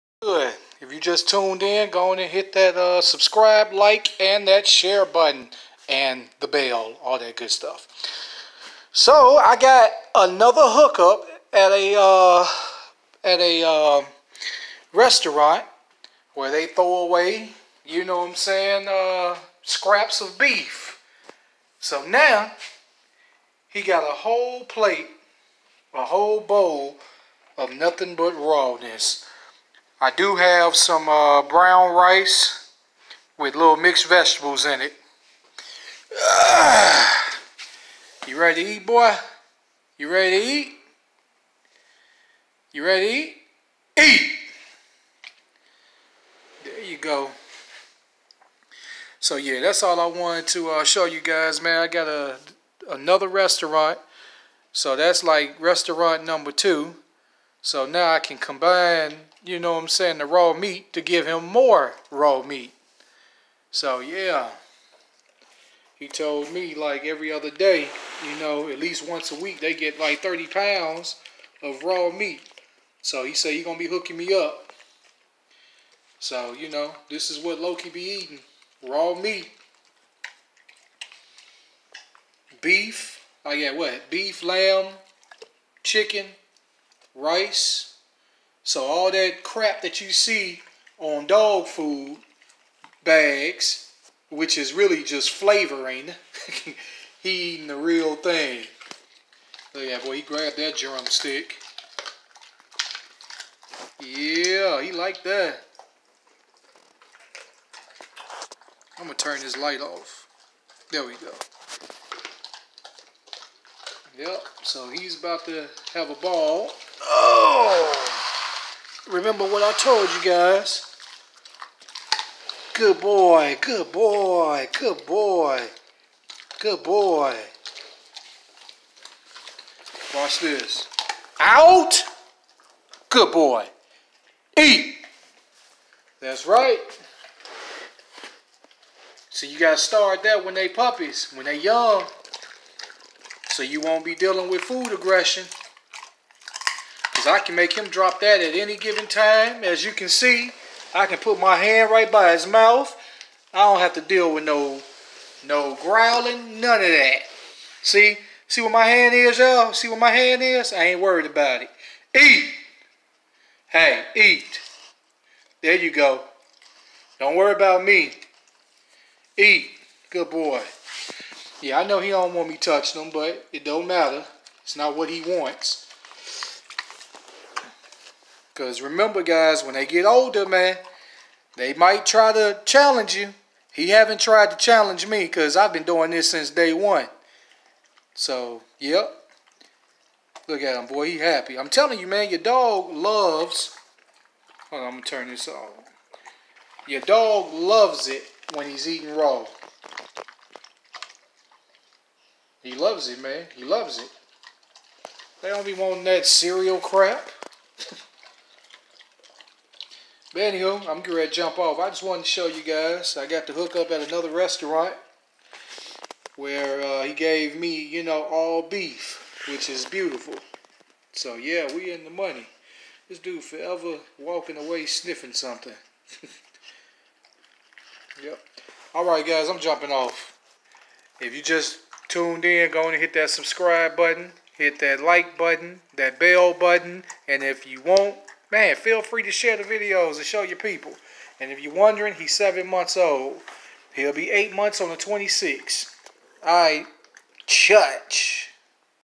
Got another restaurant giving me raw meat
got the hook up at one of my customers accounts who was literally throwing away all the scraps and I told him bro let me get that meat up off you guys. I showed him a video of Loki eating raw and he was happy to give it to me